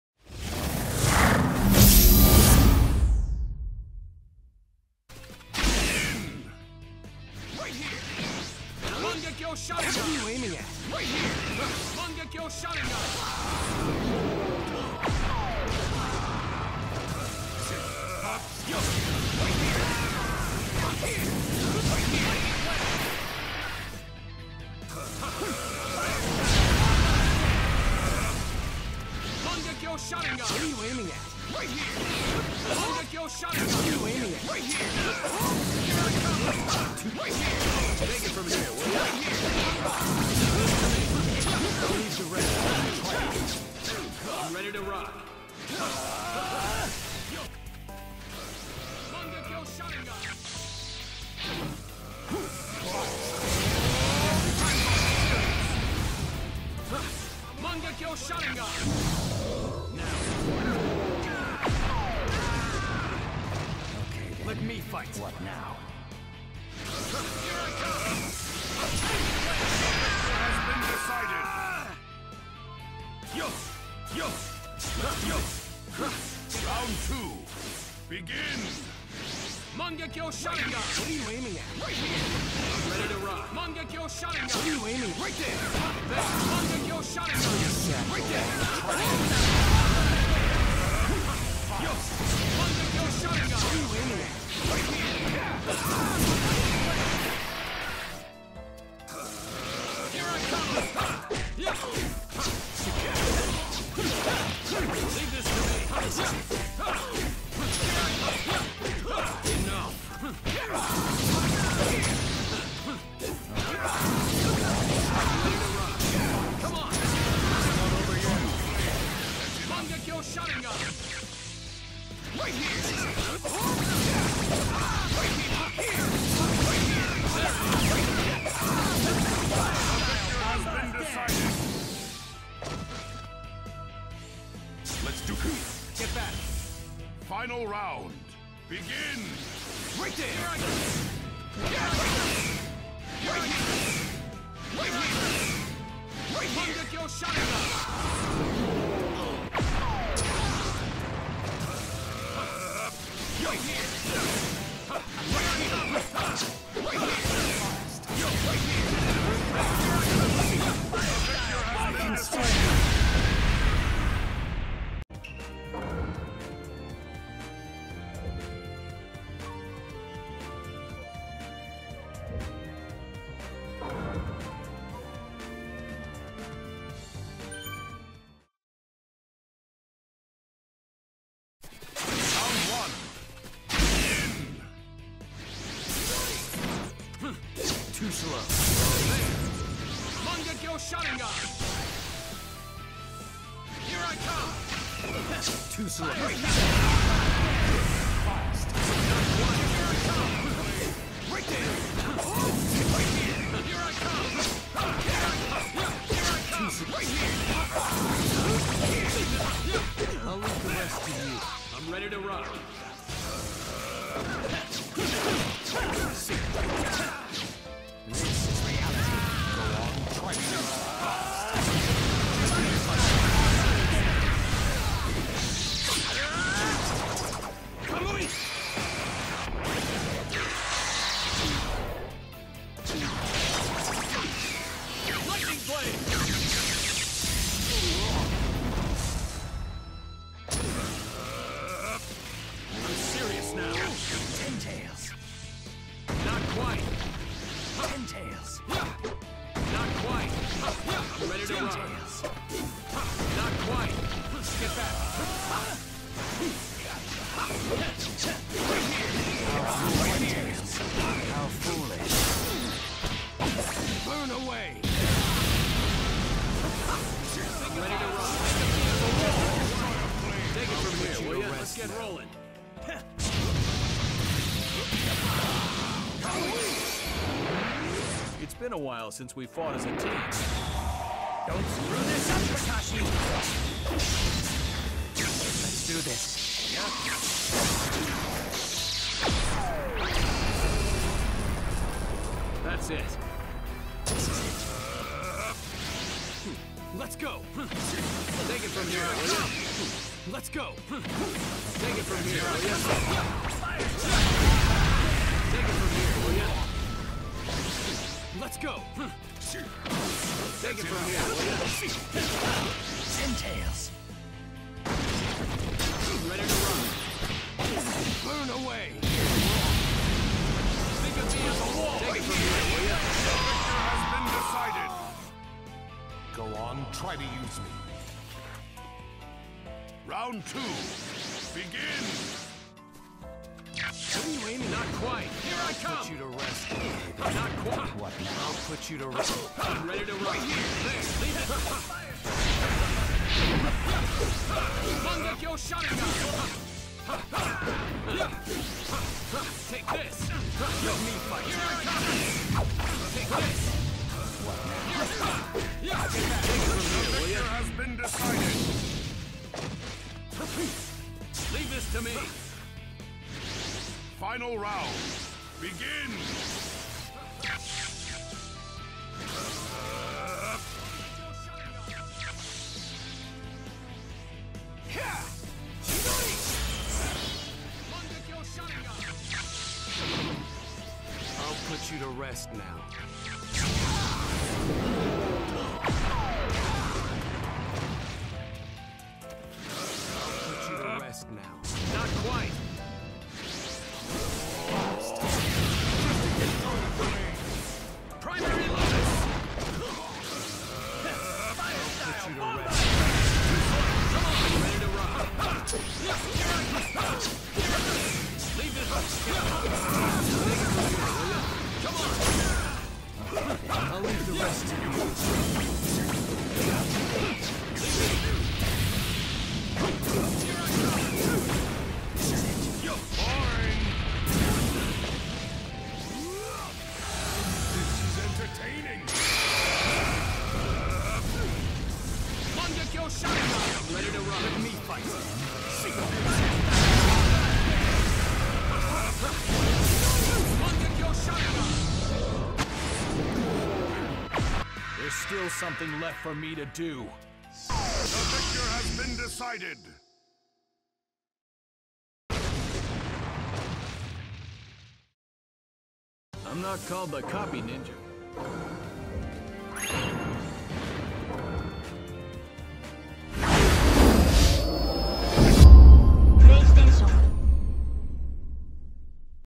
Kakashi's Crazy New Jutsu!
kakashi kamui mixed with lightning speed!